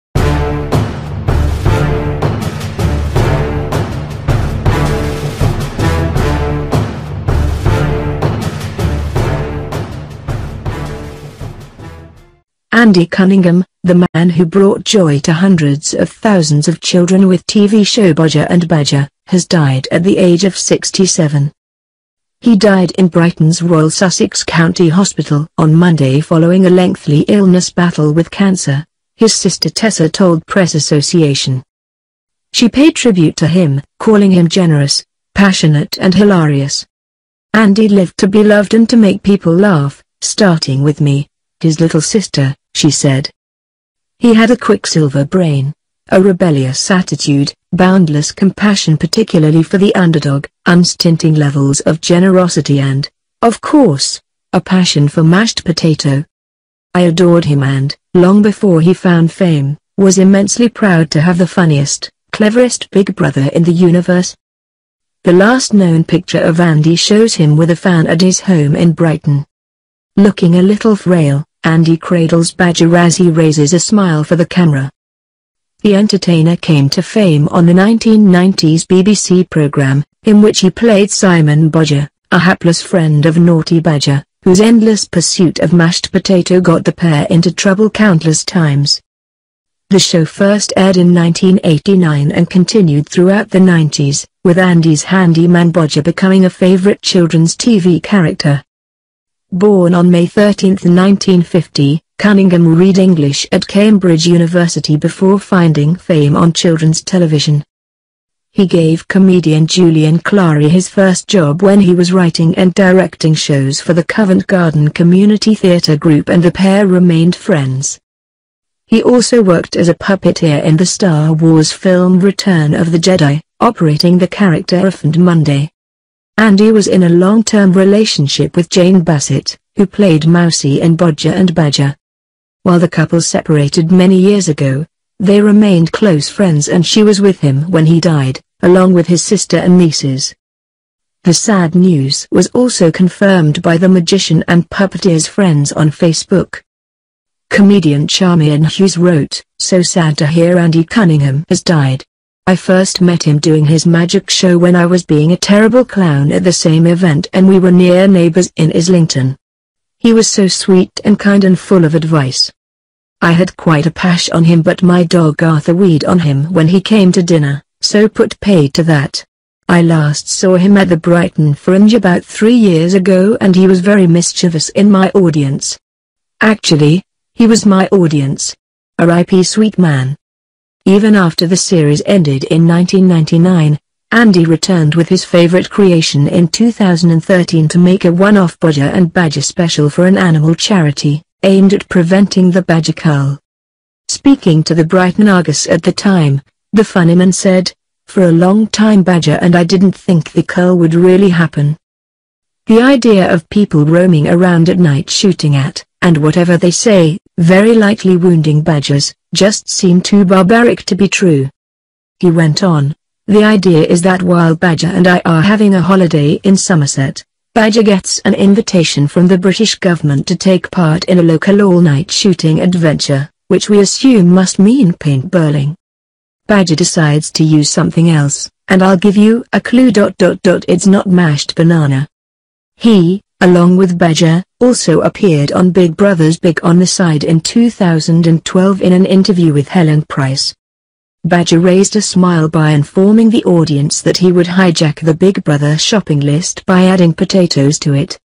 Andy Cunningham, star of BBC's Bodger And Badger, dead aged 67
Follow us on twitte @ goo.gl/tJSUG4